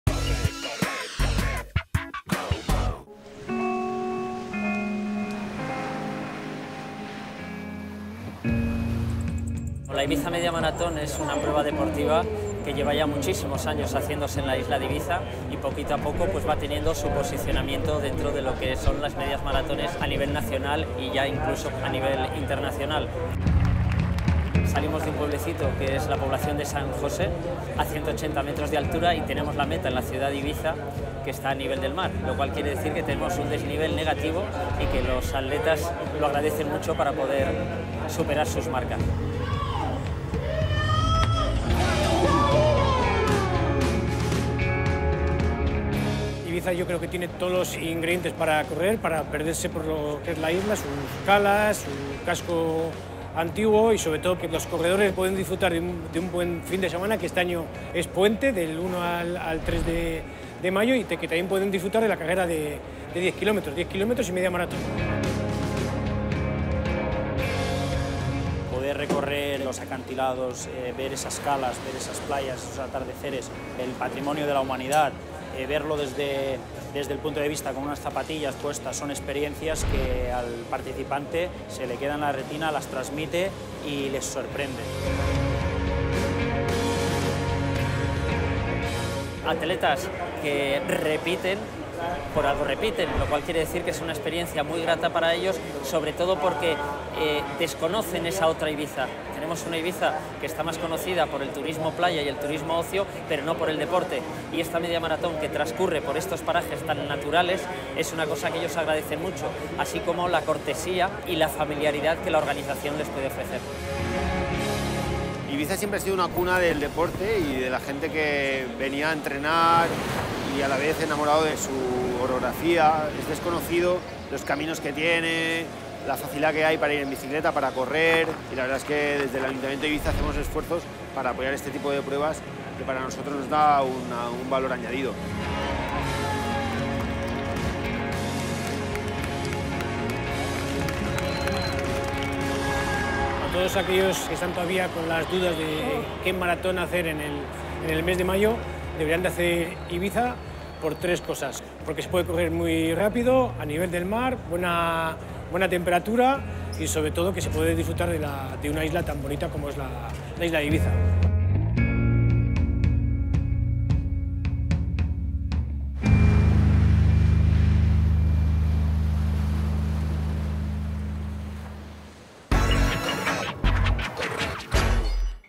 Medio Maratón de Ibiza 2020 | CORREDOR
El medio maratón de Ibiza, que tendrá lugar el 3 de Mayo, es una opción fantástica para disfrutar del mundo del running a la vez que descubrimos una de las islas con mayor encanto de nuestro país.